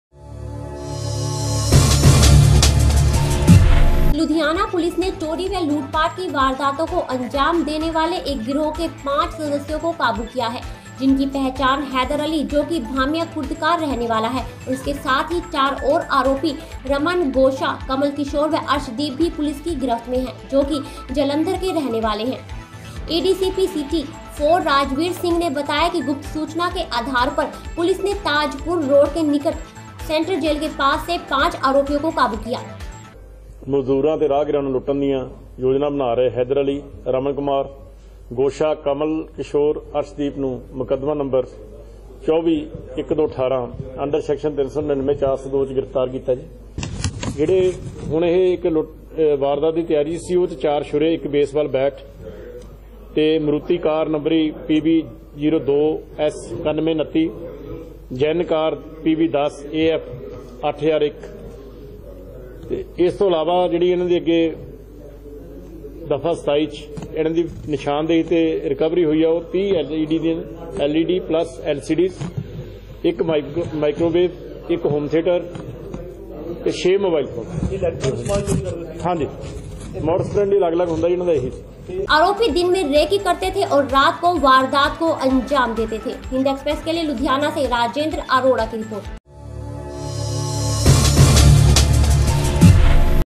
Ludhina || पुलिस के हत्थे चढ़ा चोरी व लूटपाट की वारदातों को अंजाम देने वाला गिरोह
लुधियाना (राजेंद्र अरोड़ा) : लुधियाना पुलिस ने चोरी व लूटपाट की वारदातों को अंजाम देने वाले एक गिरोह के 5 सदस्यों को काबू किया है। जिनकी पहचान हैदर अली जोकि भामियाँ खुर्द क रहने वाला है और इसके साथ ही चार और आरोपी रमन, गोशा, कमल किशोर व अर्शदीप भी पुलिस की गिरफ्त में हैं, जोकि जालन्धर के रहने वाले हैं।

एडीसीपी सिटी-4 राजवीर सिंह ने बताया, कि गुप्त सूचना के आधार पर पुलिस ने ताजपुर रोड के निकट सेंट्रल जेल के पास से 5 आरोपियों को काबू किया। छानबीन के दौरान आरोपियों से 4 तेजधार हथियार, एक बेसबाल बैट, 30 एलसीडी/एलईडी, 1 माइक्रोवेव, 1 होम थिएटर, 6 मोबाइल फोन, एक मारुति व एक जेन कार बरामद हुई हैं। इन आरोपियों के काबू में आने व बरामद समान से पुलिस ने लुधियाना सहित फिलोर, जालंधर, होशियारपुर, फगवाड़ा इत्यादि क्षेत्रों में हुई कुछ वारदातों को सुलझा लिया है। आरोपियों पर पहले भी कई आपराधिक मामले दर्ज हैं।

एडीसीपी ने बताया, कि गिरोह का सरगना हैदर अली जब जालंधर की जेल में बंद था, तो उसकी अन्य आरोपियों से मुलाकात हुई, जिन्होंने जेल से बाहर निकल कर गिरोह बना लिया। बाइट। आरोपी दिन में रेकी करते थे और रात को वारदात को अंजाम देते थे।